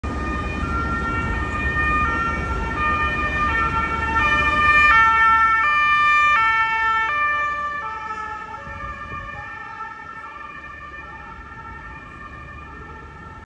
Einsatzfahrt FuStW Luzerner Polizei in der Stadt Luzern
Ein Funkstreifenwagen der Luzerner Polizei auf Einsatzfahrt in der Stadt Luzern. (Schweiz)